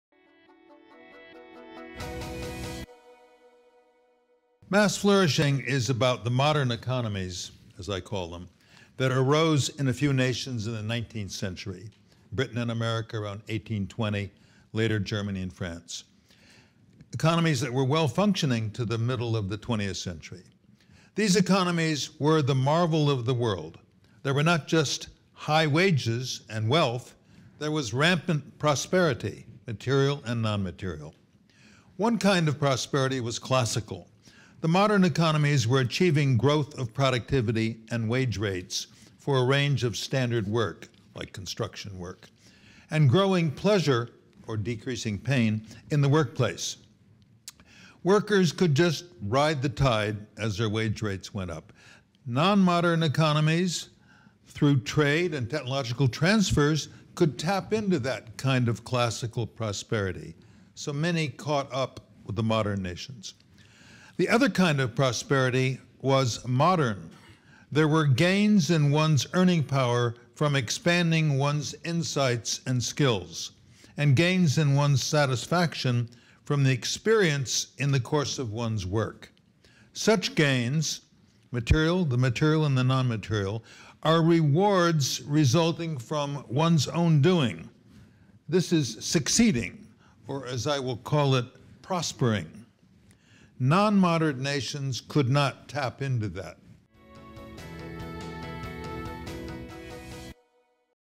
Two Kinds of Prosperity
Nobel Laureate Edmund Phelps, author of "Mass Flourishing," discusses the two kinds of prosperity that arose in the 19th century.

This Carnegie Council event took place on October 28, 2013. For complete audio and transcript and video clips, go